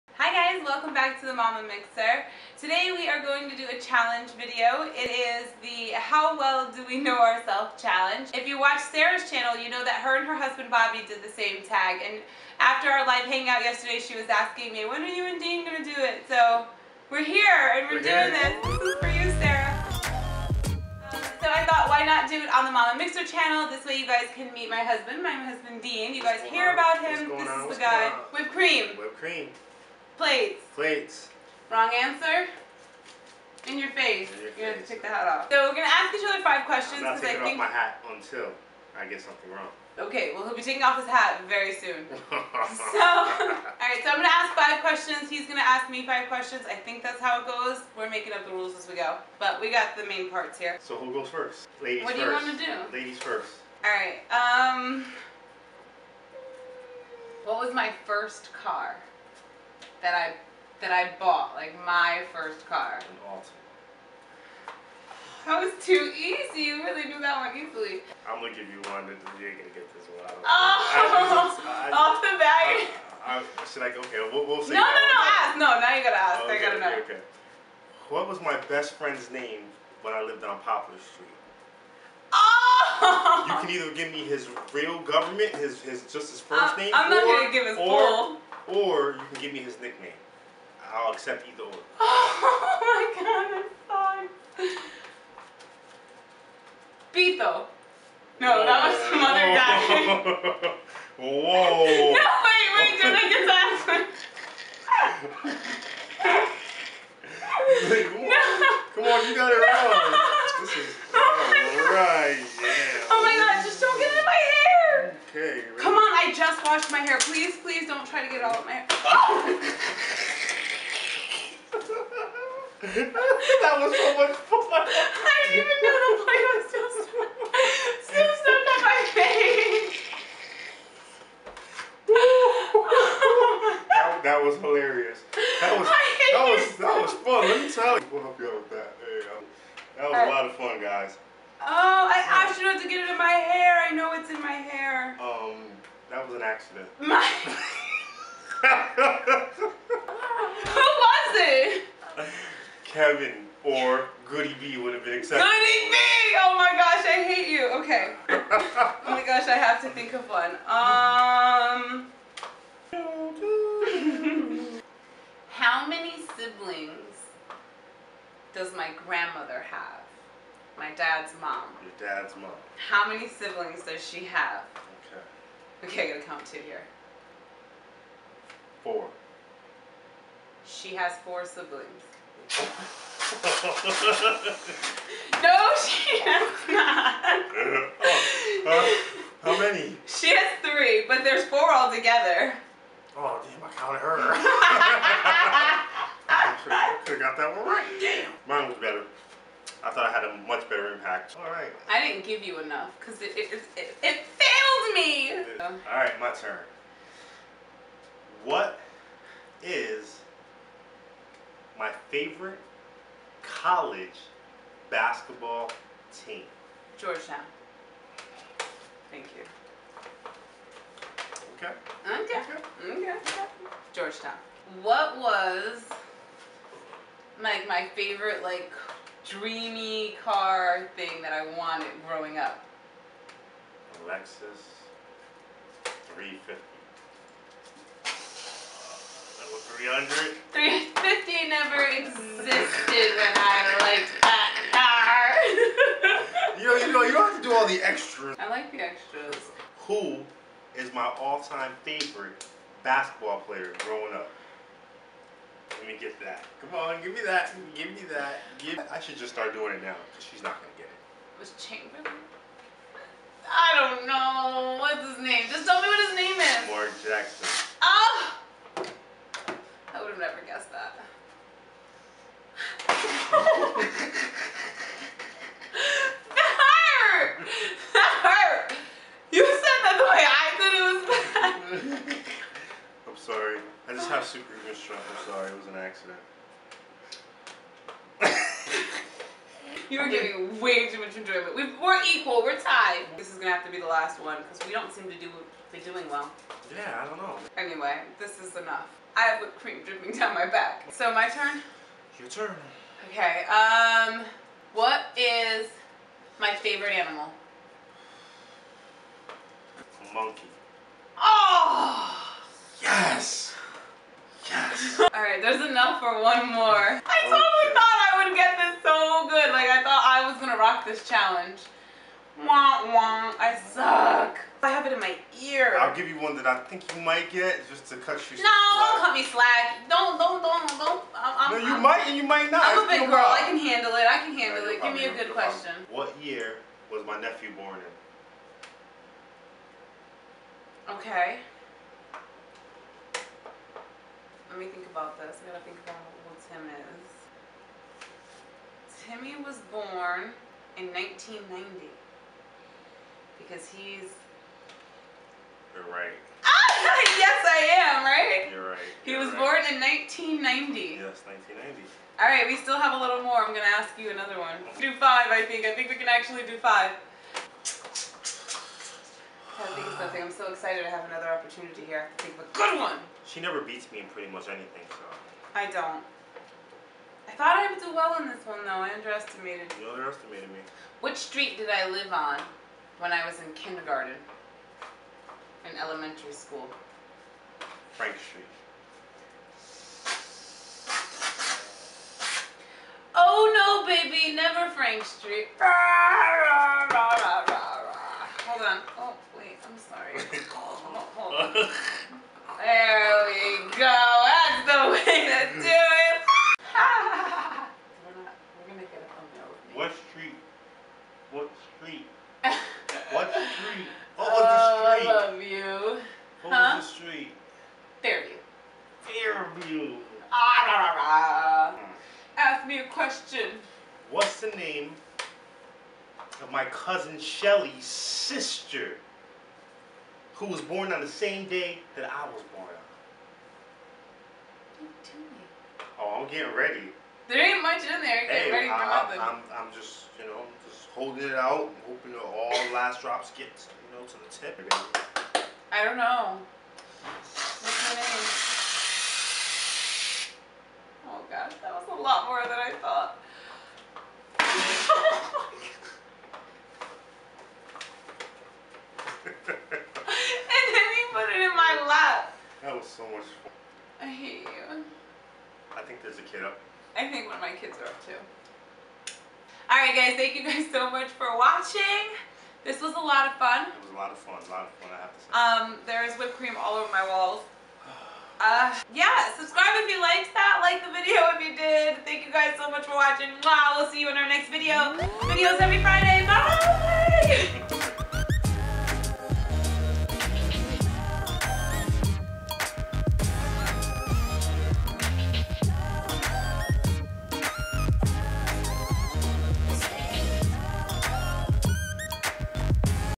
HOW WELL DO WE KNOW EACH OTHER | MEET MY HUSBAND | THE MAMA MIXER
Hey friends! Today Shirlee from Life as a Twin Mom are doing the How Well Do We Know Each Other Challenge with her husband Dean. Watch this weeks video to see who creams who!